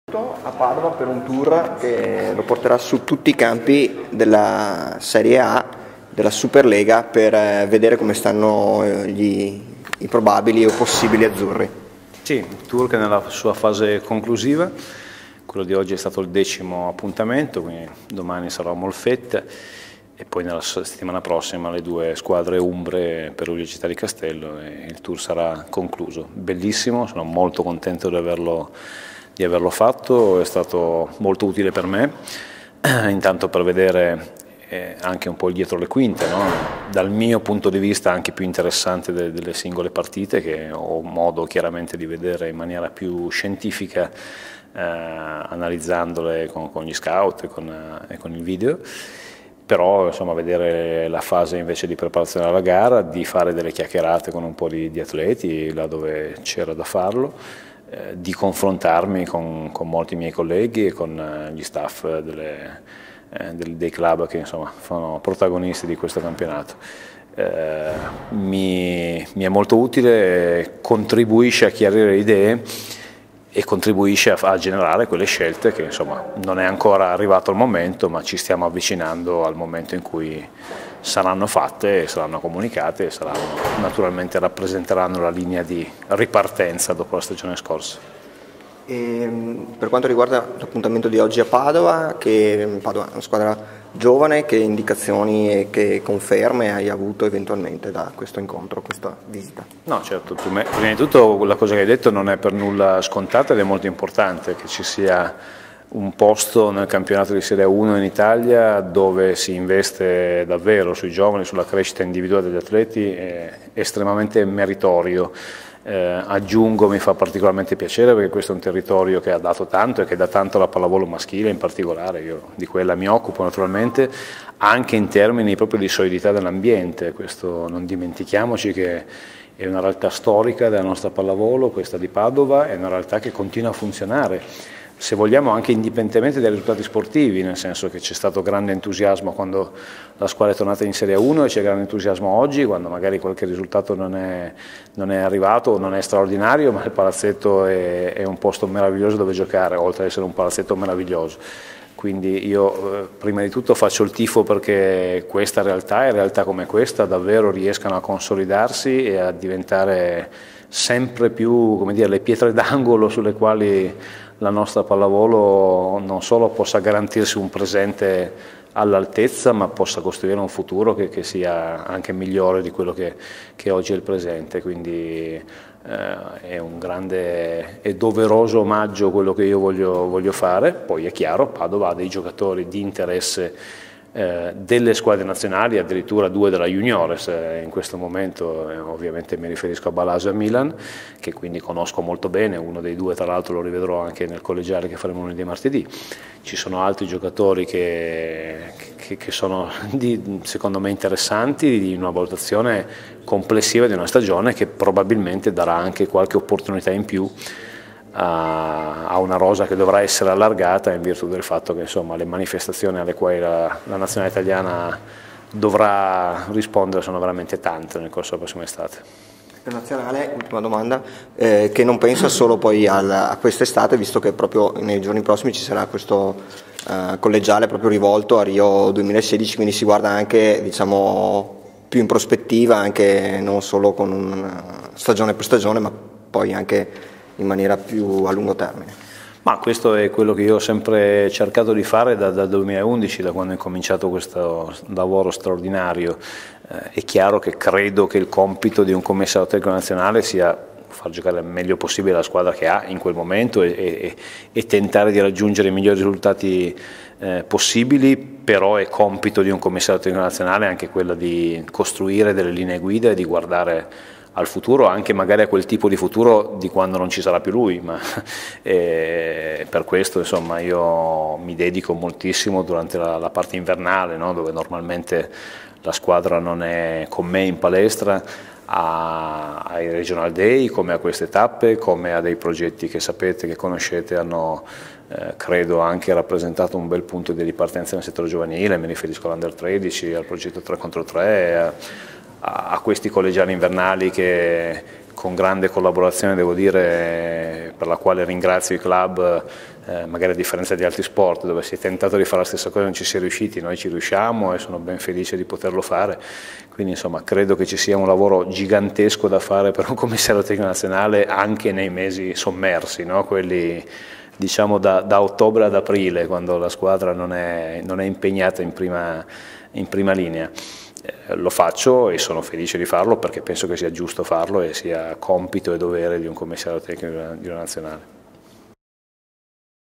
Le parole di Mauro Berruto a Padova
L'intervista a Mauro Berruto - coach della Nazionale italiana di pallavolo maschile - giunto in Veneto per seguire l'allenamento della Tonazzo Padova. In questa intervista le sue parole sulla squadra e sul progetto della Nazionale in vista di Rio 2016.